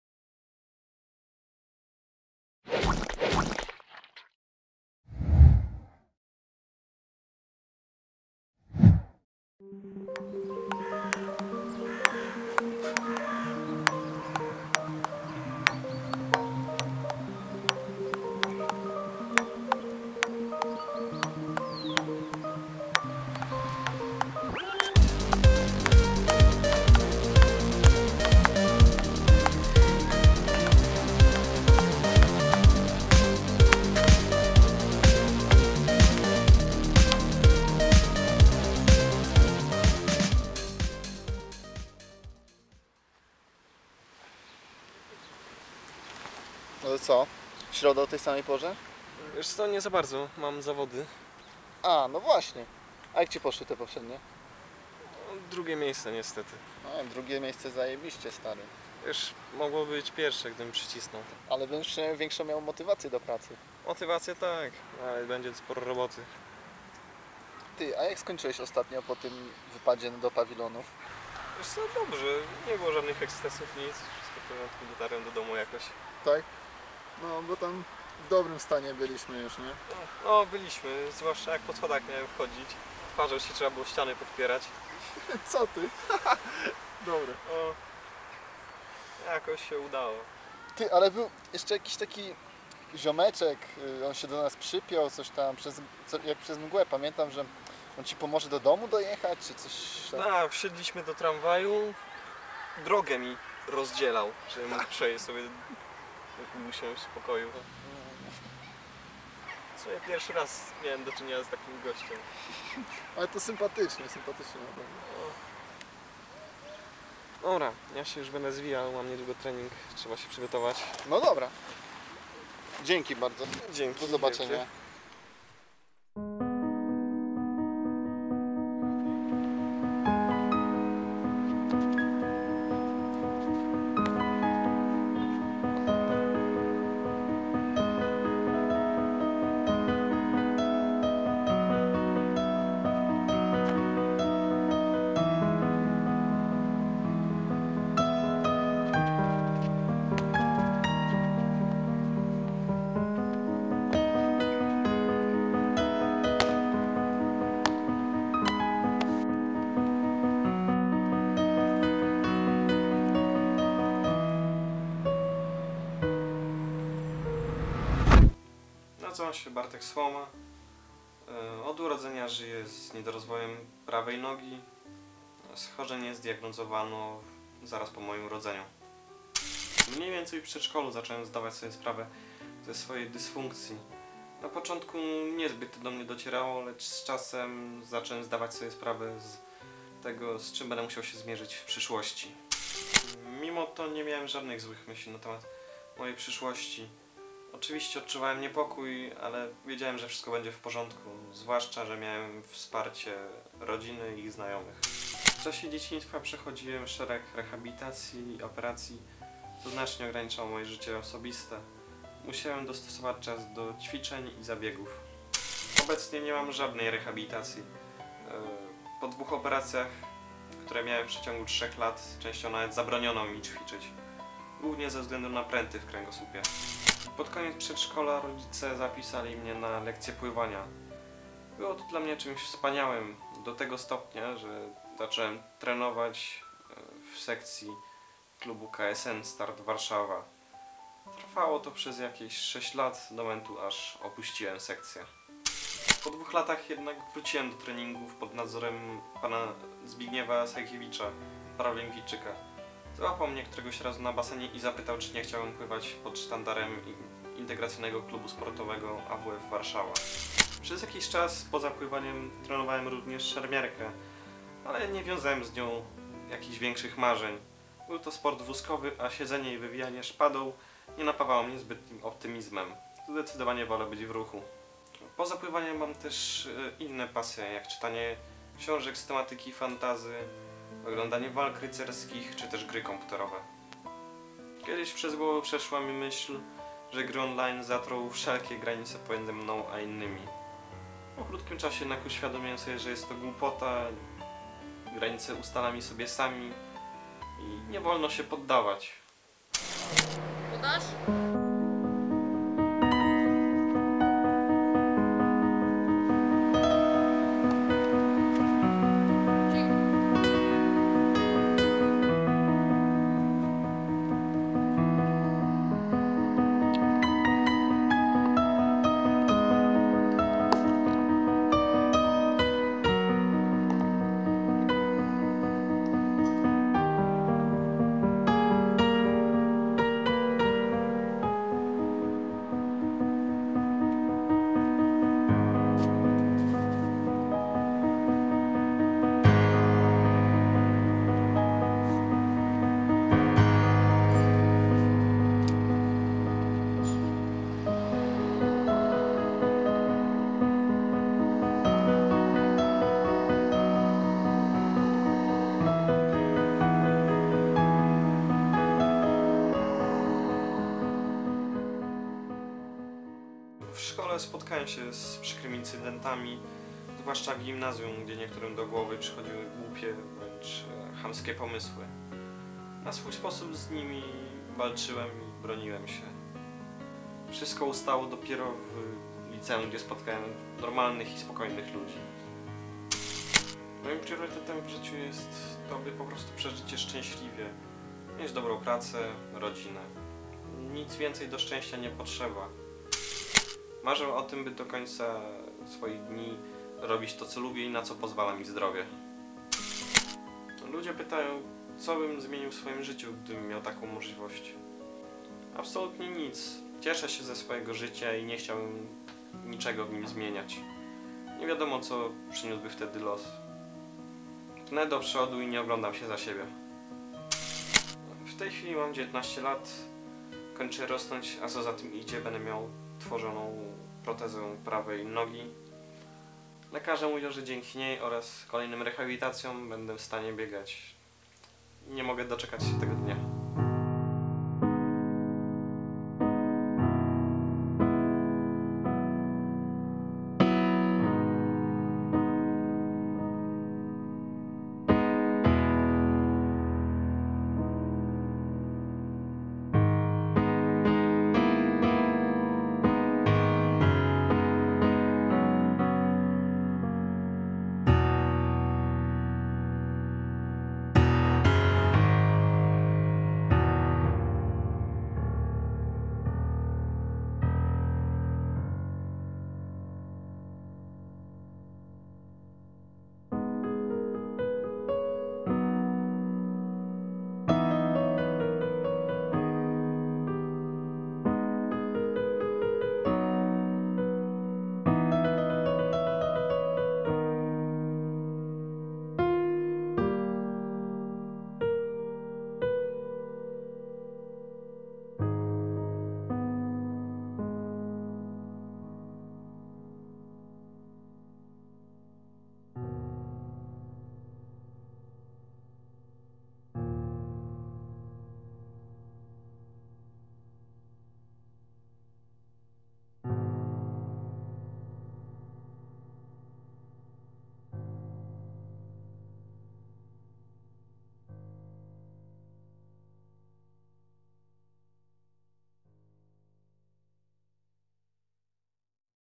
BARTEK
Dokument opowiadający historię Bartka, chłopaka, dla którego wymówki nie istnieją.

Występują:
Bartłomiej Słoma
Marek Podgórski
Daniel Skurczyński
Jakub Niewiadomski

Scenariusz:
Adam Pepłoński

Reżyseria
Blerim Ukići

Zdjęcia:
Blerim Ukići

Postprodukcja:
Adam Pepłoński
Blerim Ukići